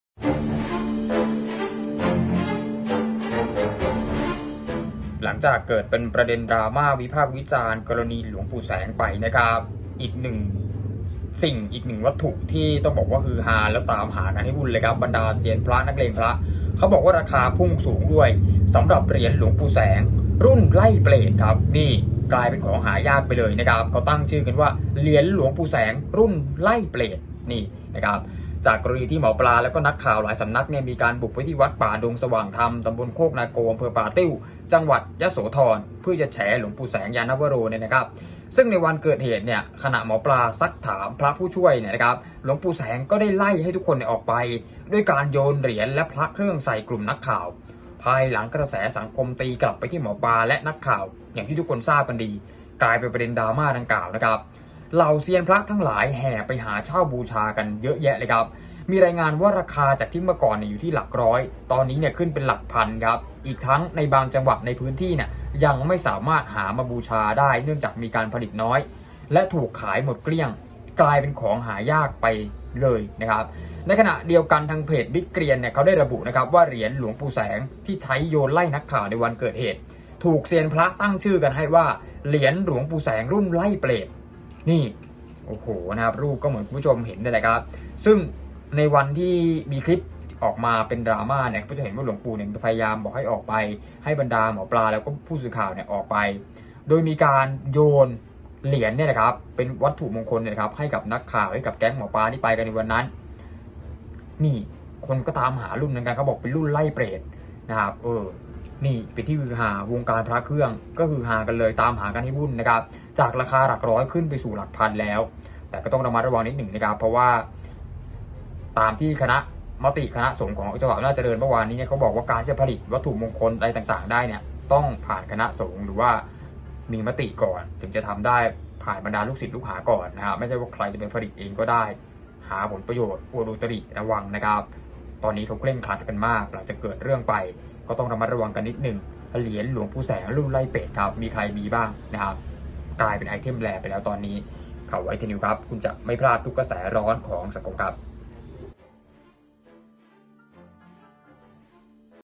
แห่เช่า! เหรียญ หลวงปู่แสง หมดเกลี้ยงทุกแผง เซียนพระหาวุ่น "รุ่นไล่เปรต"| Thainews - ไทยนิวส์
ราคาพุ่งสูง!! เซียนพระแห่หาเช่า เหรียญหลวงปู่แสง กลายเป็นของหายาก ล่าสุดถูกเซียนพระ ตั้งชื่อให้ว่า เหรียญหลวงปู่แสงรุ่นไล่เปรต​